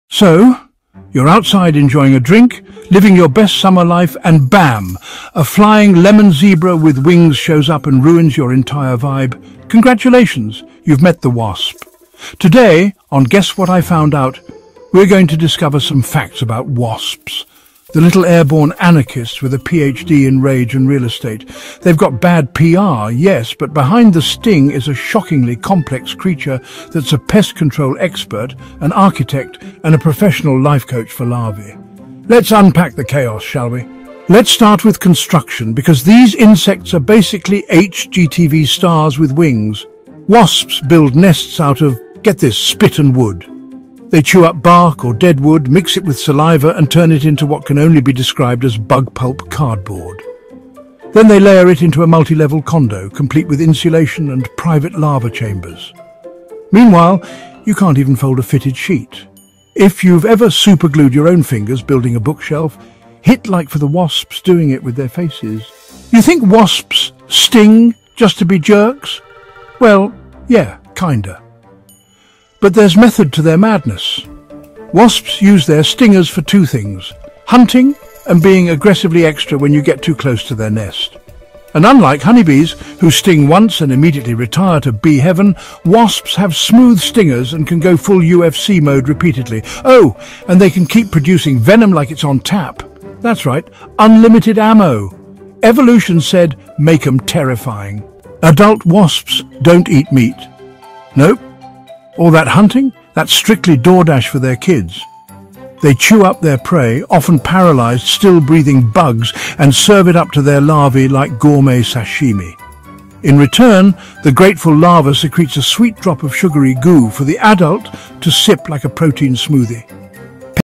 WASPS – Here’s Why They Secretly Run the Planet
Why are wasps so terrifyingly extra? In this episode of “Guess What I Found Out,” we dive into the weird world of wasps—those buzzing backyard villains who are secretly master builders, pest control pros, and surprisingly good parents (in a horrifying way).
Learn how wasps sting repeatedly, build paper condos with their faces, and perform surgical bug assassinations—all while pollinating flowers and saving crops. From social wasps to solitary giants like tarantula hawks, it’s time to give these misunderstood maniacs their due.